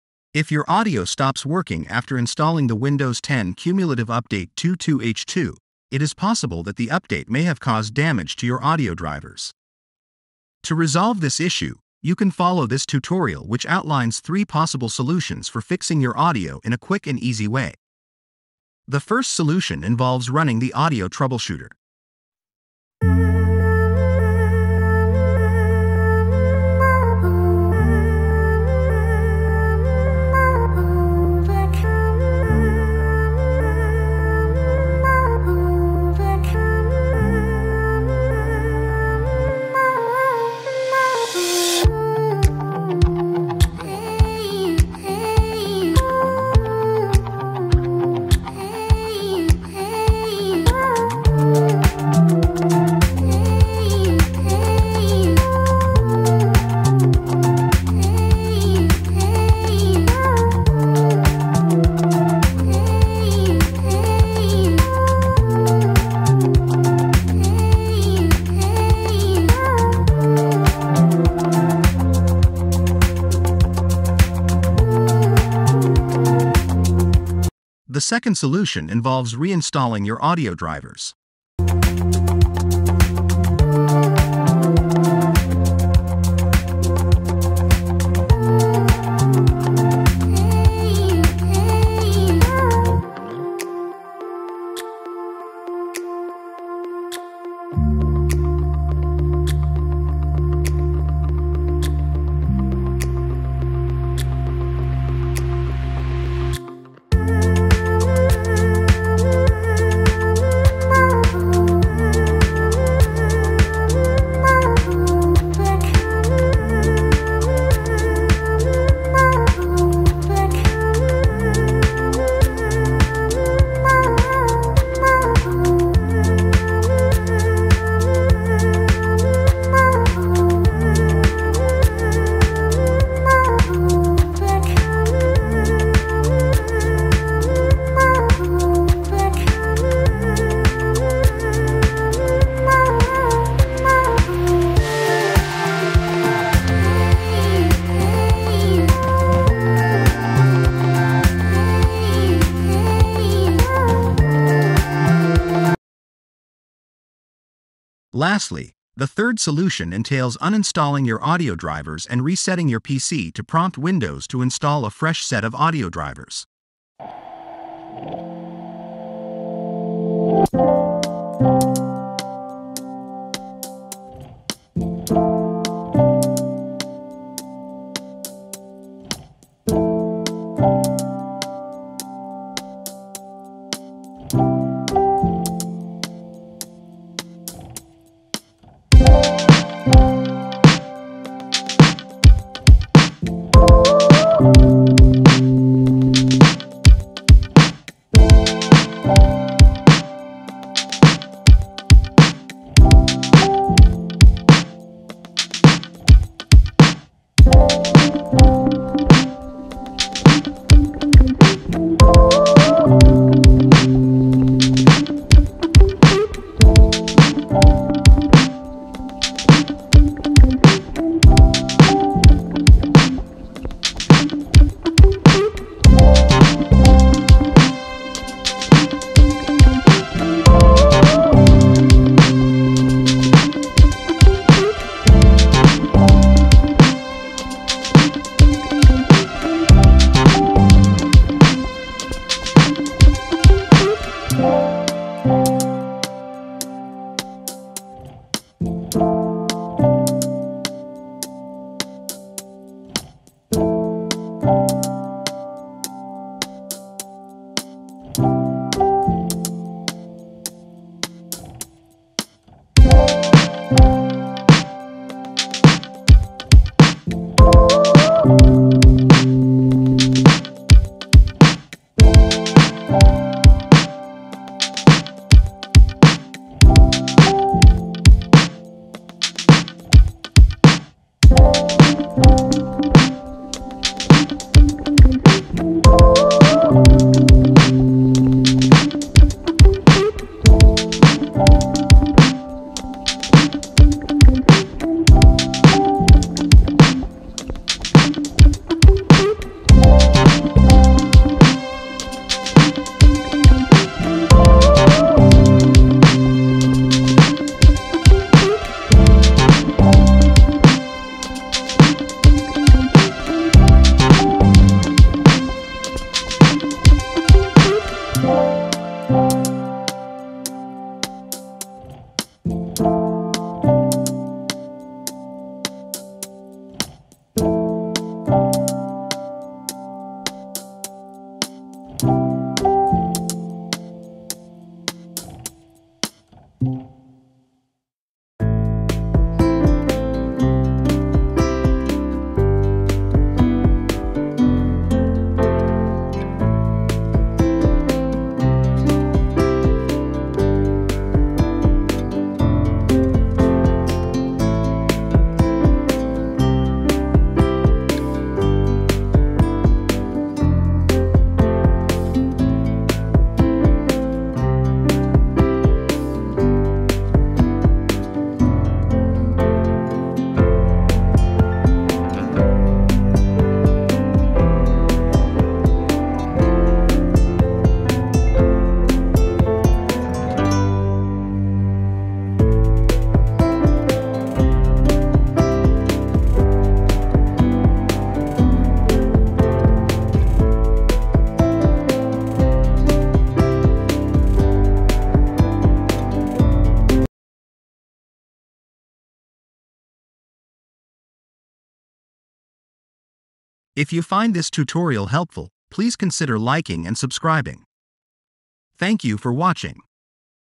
Audio Not Working After 22H2 Update Windows 10
If you install the Windows 10 cumulative update 22H2 and your audio is no longer working. this could be happening because Windows update damaged your audio drivers so in this tutorial you will learn how to fix your audio in a quick and easy way.

No audio coming out of speakers and headphones after Windows 10 22H2 update , it works fine in my other devices (phone/tab).